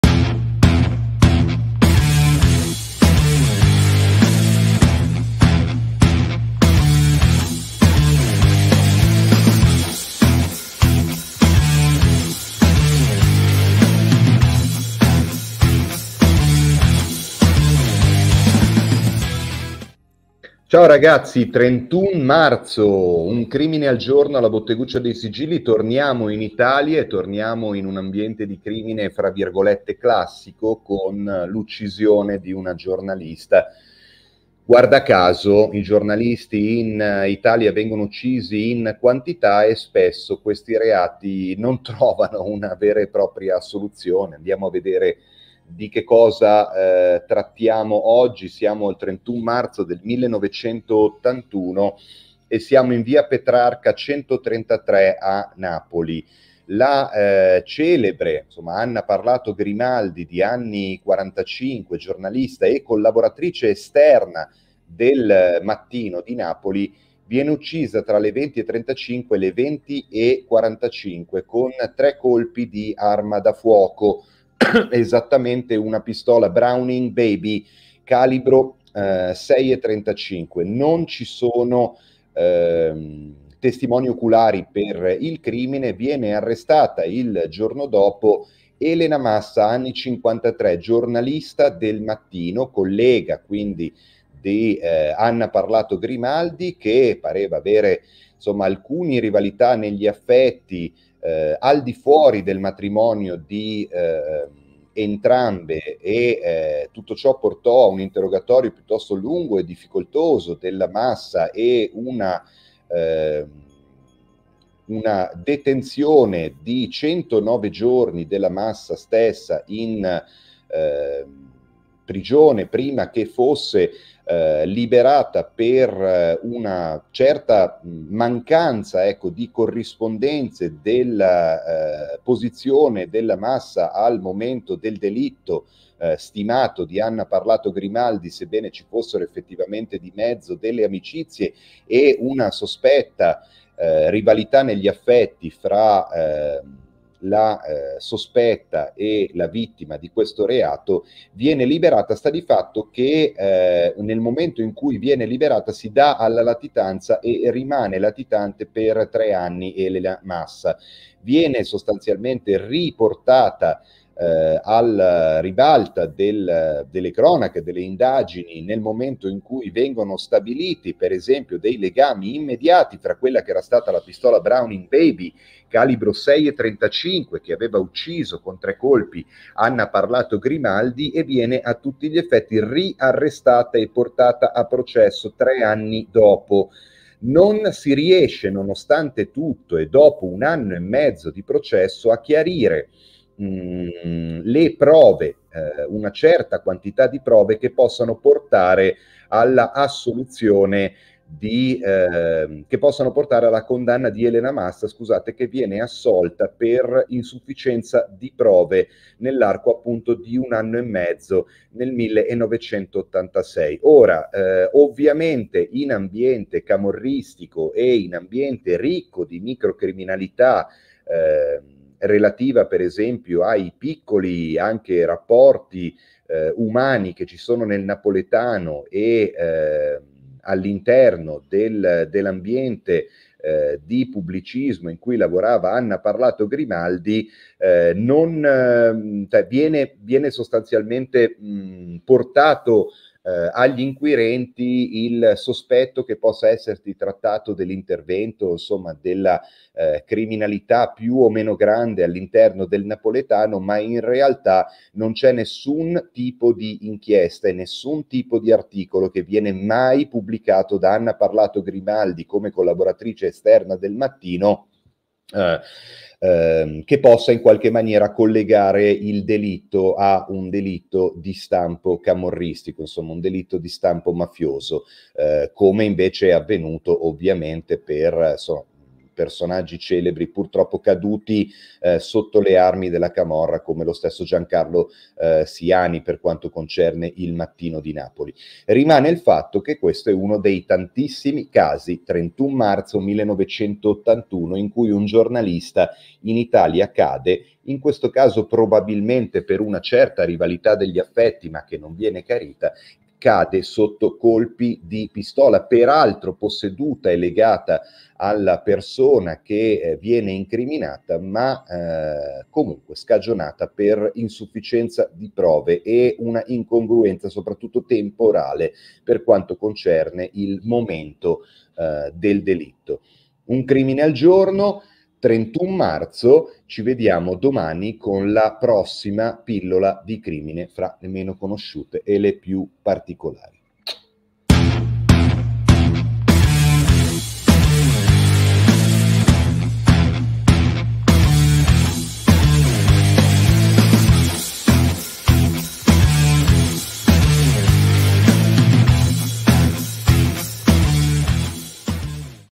Meno Giornalisti, più Impunità
UN CRIMINE AL GIORNO, 31 marzo - Non si sa com'è, ma giornalisti e giornaliste sono una specie incredibilmente mortale in Italia - senza mai uno straccio di condanna o prova...

«Un crimine al giorno», Pillole di crimine uniche giorno per giorno su @crackupart